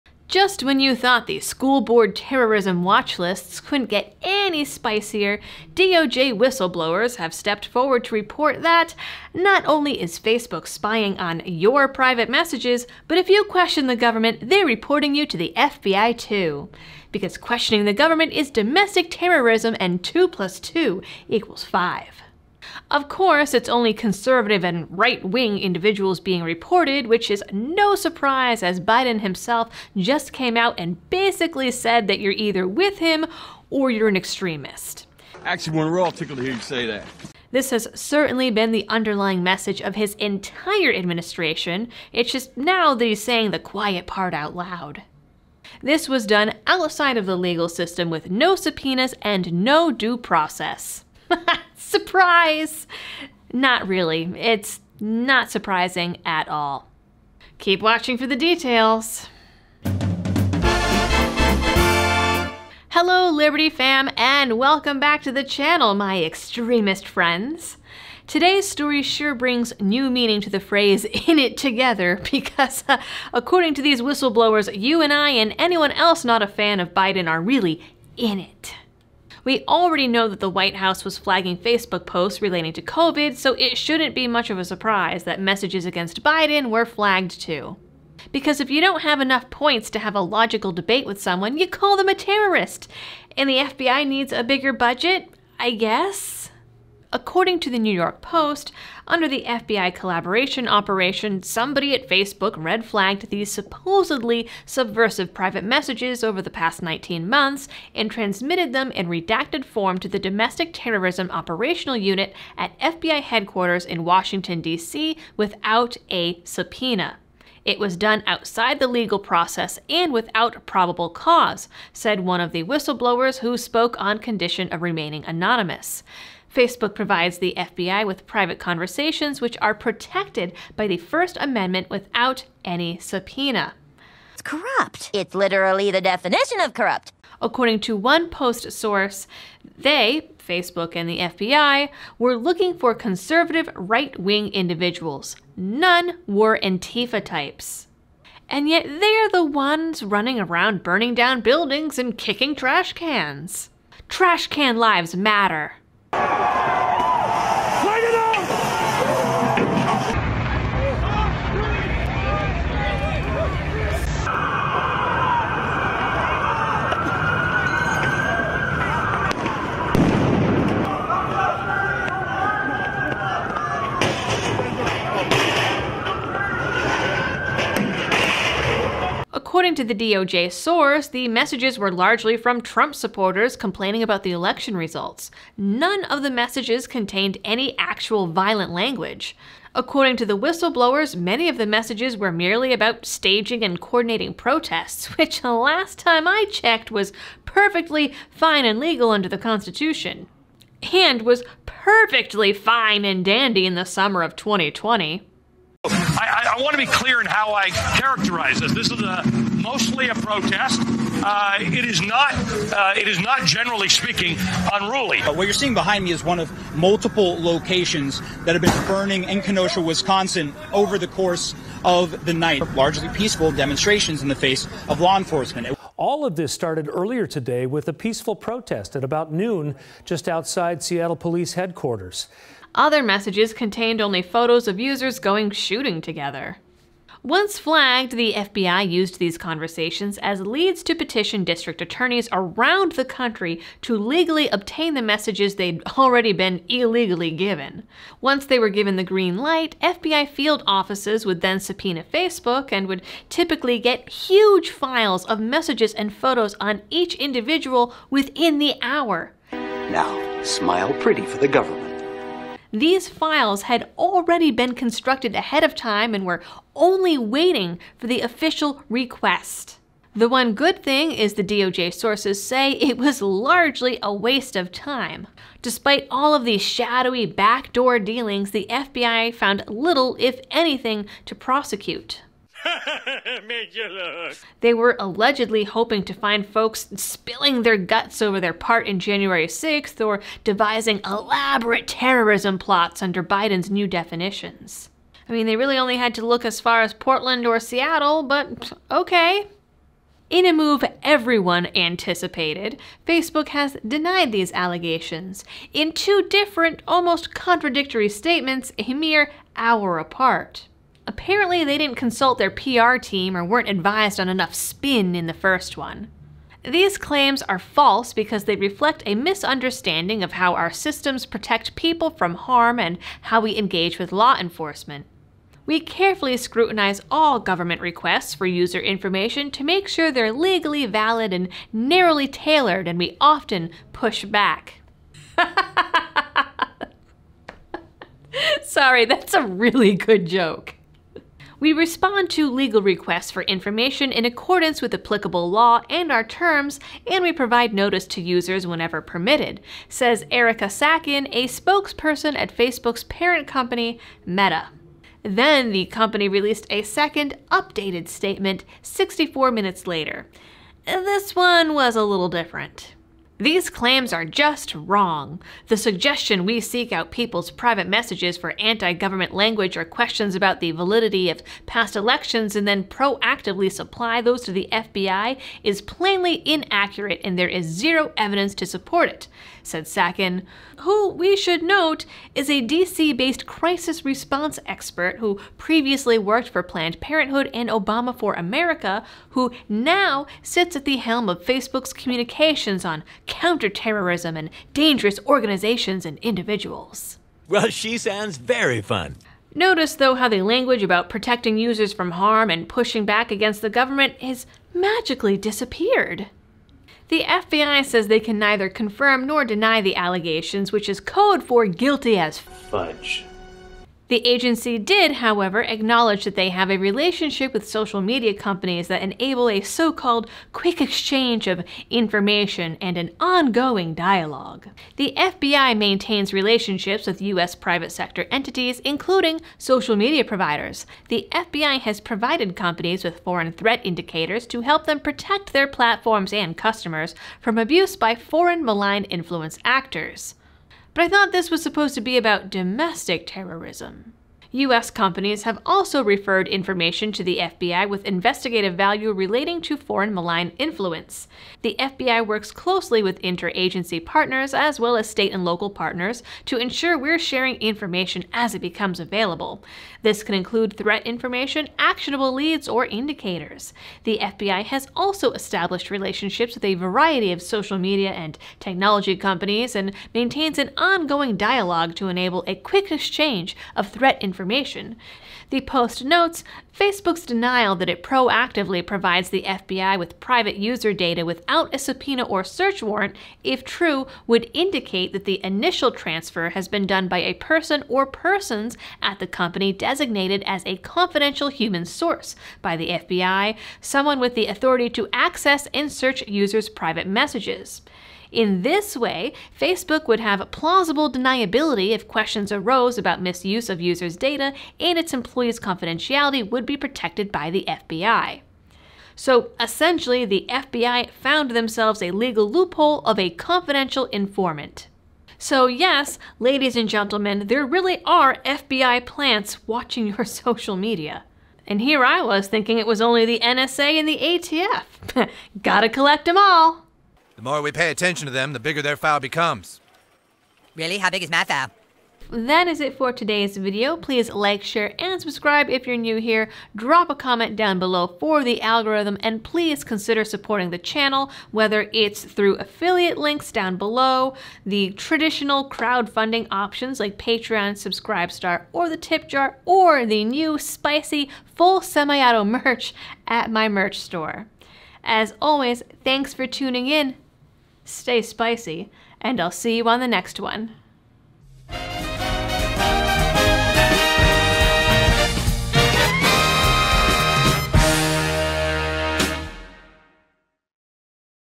Facebook Spying, Reporting Conservatives to the FBI
FBI whistleblowers say Facebook has been spying on conservatives and reporting them to the FBI as domestic terr*rists.

SUPPORT
Bitcoin: 3FKmZtjgaPAFz3YGvDAdZVnBhA6TF9HDk5
Litecoin: MSw6yRktpJK7NnJZFE9jgozHbGWjJ7tsas
Recommended books, equipment, and more

OTHER PLATFORMS
Twitter, Instagram, and Facebook: @thelibertydoll